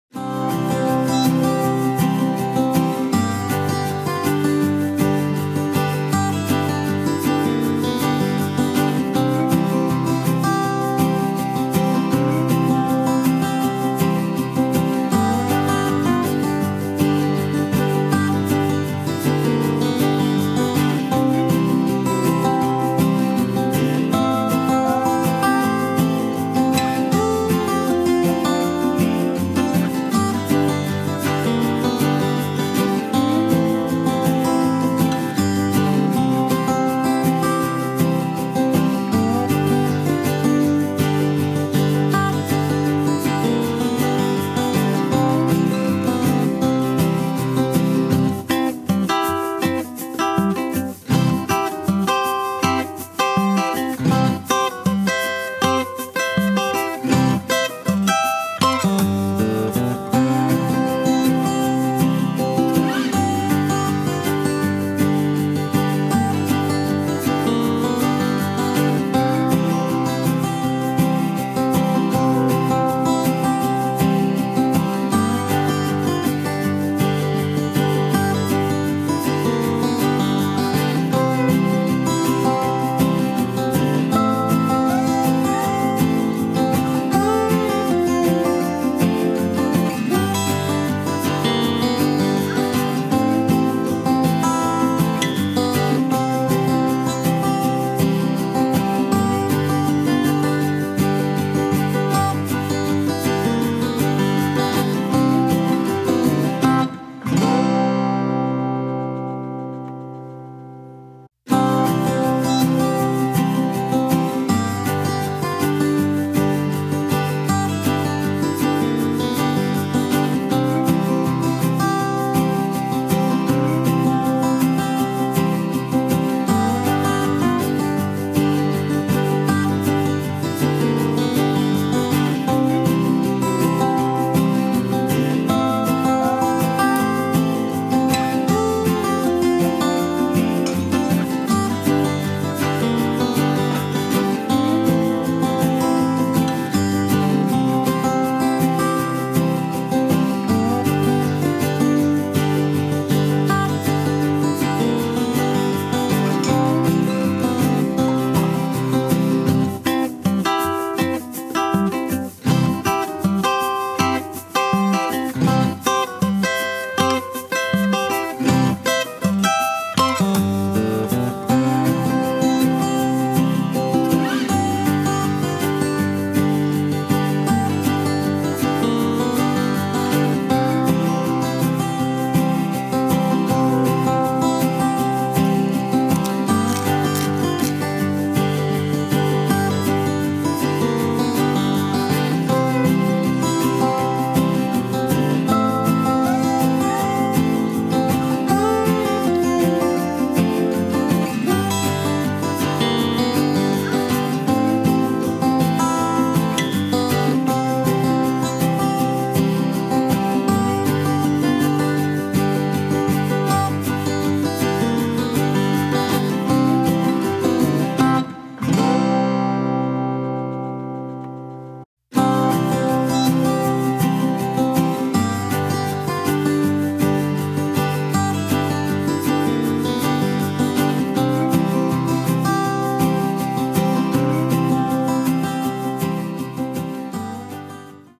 Mandarin Date Chutney | No Sugar | No Preservatives | Quick and Easy | Great use of excess produce
Chutney, a versatile savoury condiment, is accompanied to complement/enhance the flavour of main dish. Fresh and seasonal fruits/vegetables are always best to make chutneys and citrus season is great time to experiment with new recipes. So here I present my Mandarin Date Chutney, a little sweet, a bit sour and a touch spicy; as a perfect chutney should be.